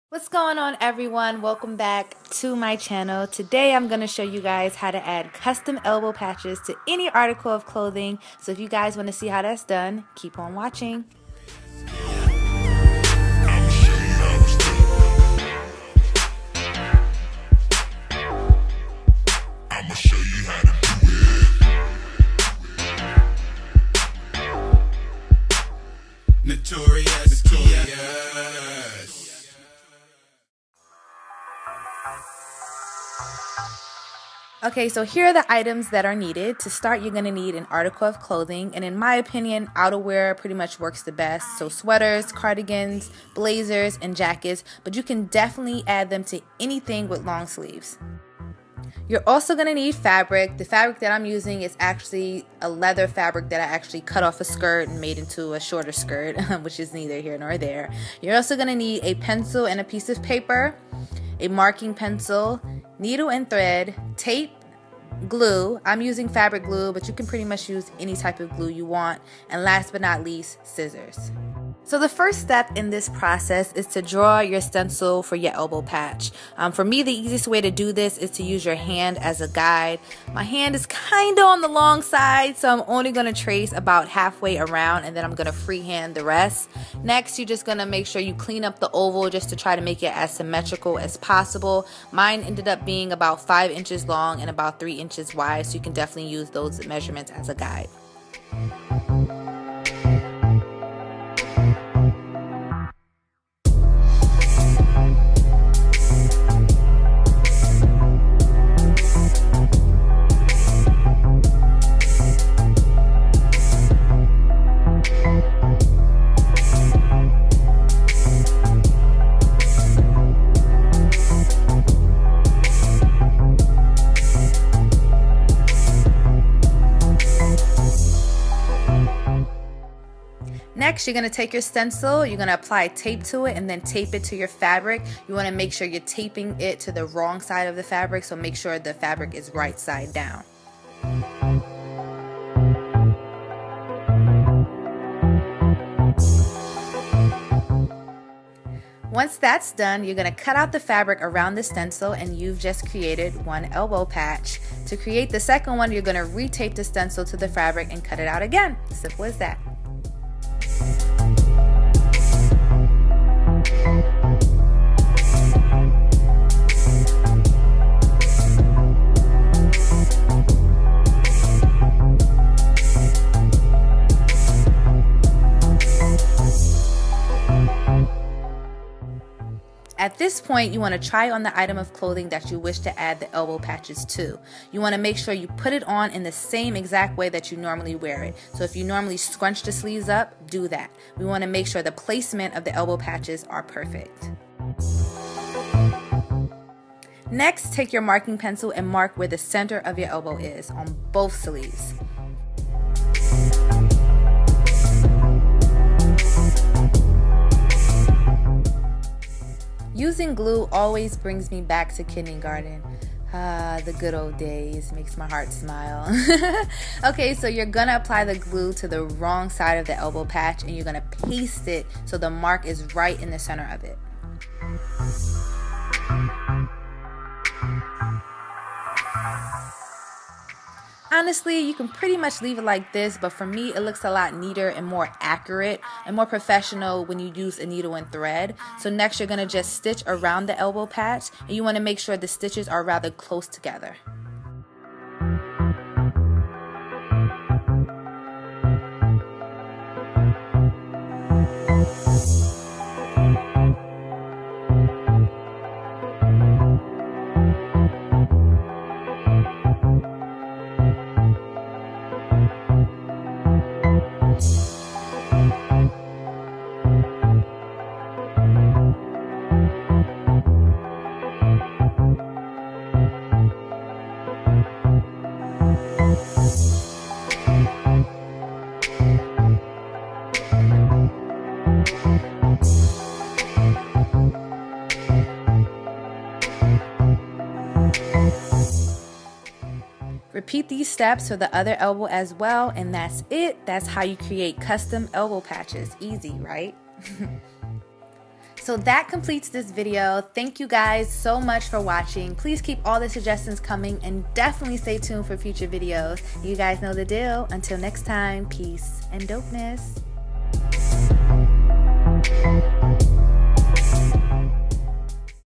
DIY Elbow Patches
Today I'm going to show you guys how to make custom elbow patches for any article of clothing. I hope you guys find this video helpful & inspiring...stay dope!

What you'll need:
-An article of clothing (preferably outerwear)
-Fabric
-Pencil & Paper
-Marking Pencil
-Tape